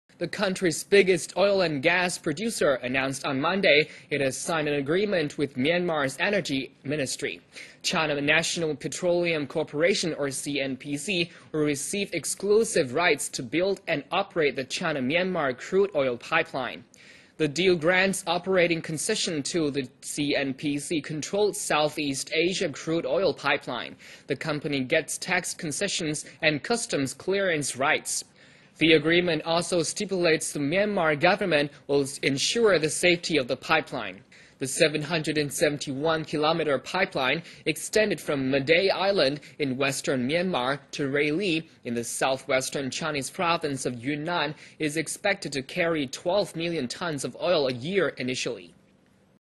China's CNPC To Build-Operate China-Mynamar Pipeline
China National Petroleum Corporation (CNPC) to receive exclusive rights to build and operate the China-Myanmar crude oil pipeline.